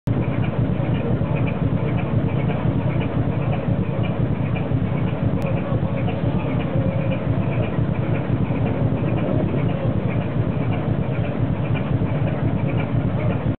Ocean Tug Holland, stationary in Terschelling/Netherlands
short trip on a summer evening